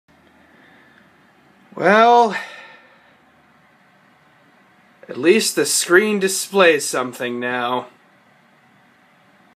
The End
Never buying a Dell again.

No, I didn't actually smash the screen, but I might as well have; the GPU is acting up again, and more than ever before.